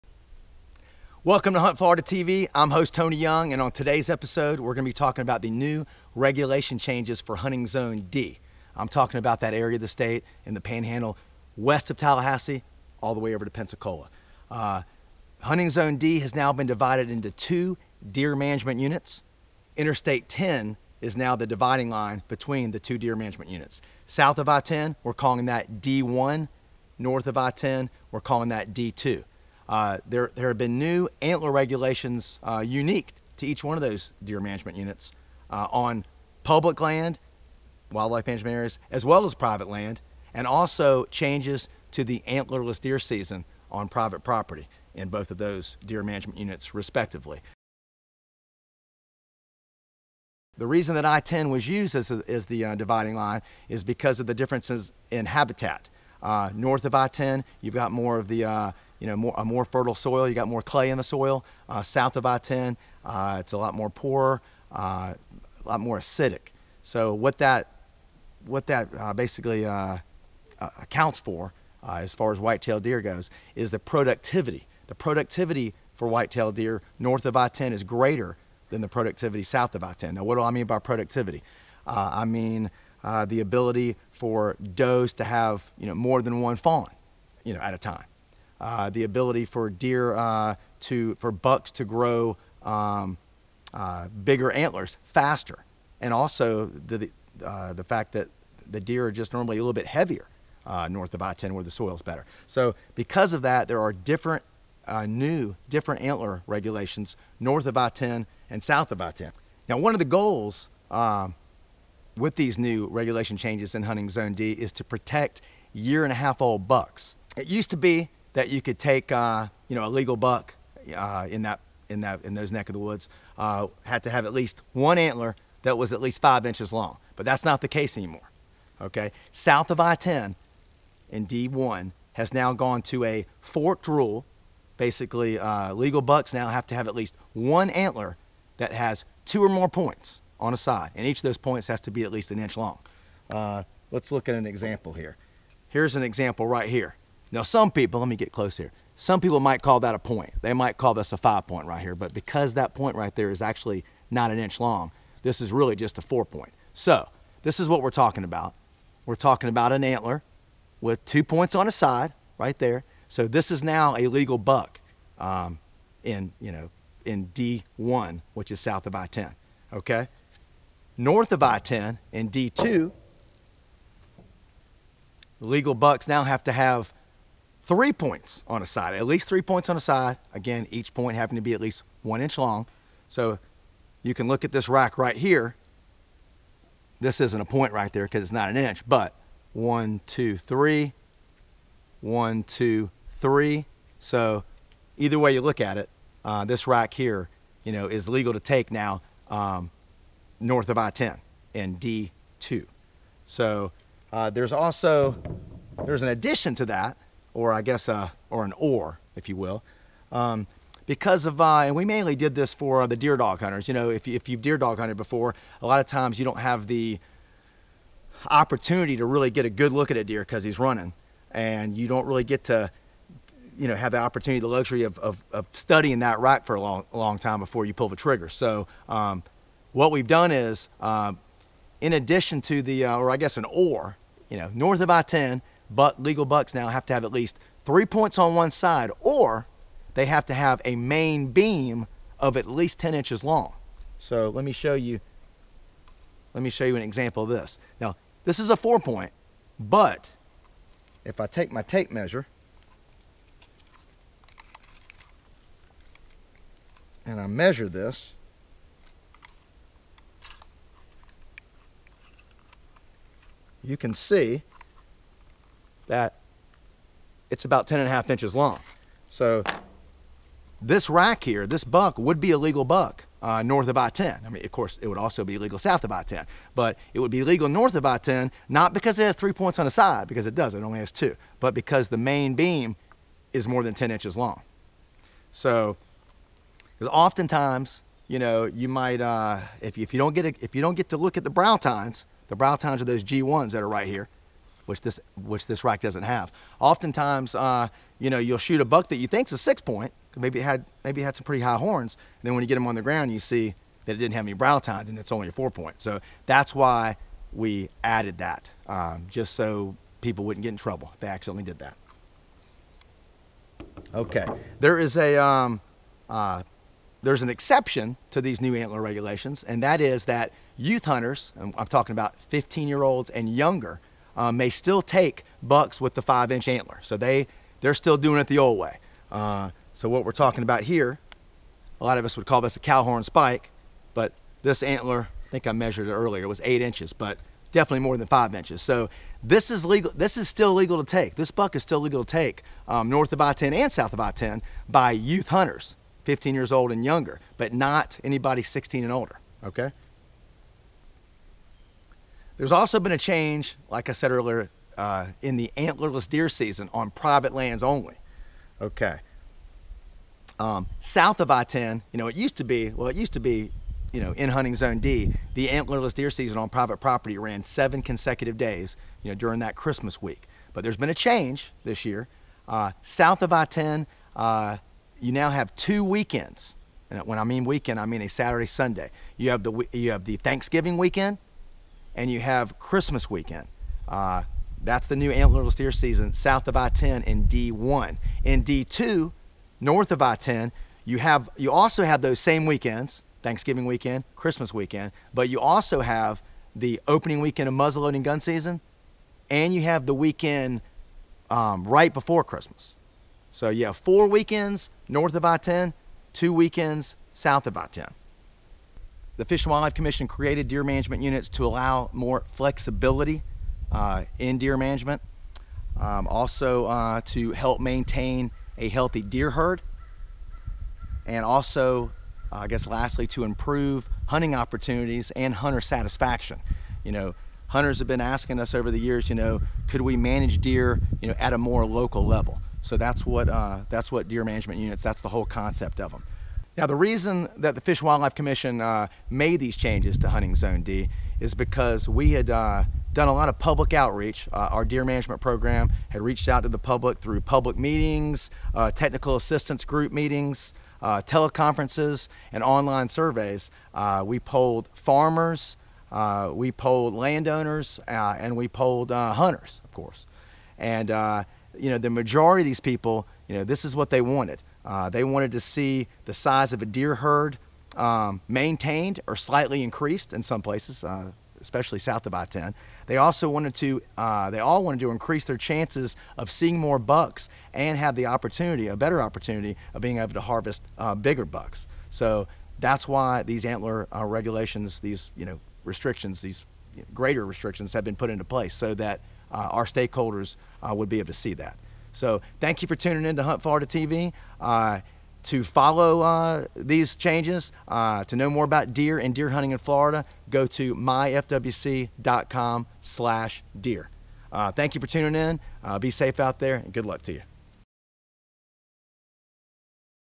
MAJOR CHANGES to deer hunting regulations in Zone D
HuntFlorida TV host Tony Young explains and illustrates the new changes to deer hunting regulations in Zone D with the creation of two deer management units, each with its own set of unique antler point rules and antlerless deer days.